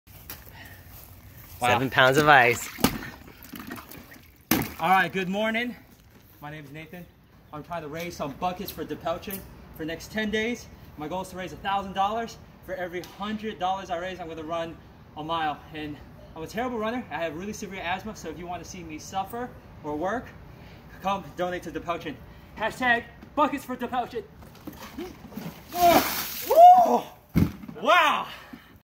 Buckets for Depelchin
Howdy Ya'll! I wanted to dedicate my birthday to raise some Buckets for Depelchin.  For the next 10 days I am trying to raise $1000 to donate to Depelchin Foster Children Center.  Let's start with the Ice Bucket Challenge! Take the challenge and spread the word about foster care. In addition, for every $100 raised towards the goal, I plan to run 1 mile.  Not everyone knows but I am a TERRIBLE runner and the struggle will be a real for me.  On October the 30th, I will record the before and aftermath of my run.  So if you want to see me work (suffer), donate to Depelchin.  Other ways to support the local organization I am involved with:  1. Pray for the children, staff, and future parents 2. Volunteer 3. Become respite providers 4. Consider becoming foster or adoption parents I challenge anyone willing to spread the word to post a video/picture of them getting soaked in ice and water. Break out the coolers! BucketsforDepelchin For Jesus, For Good.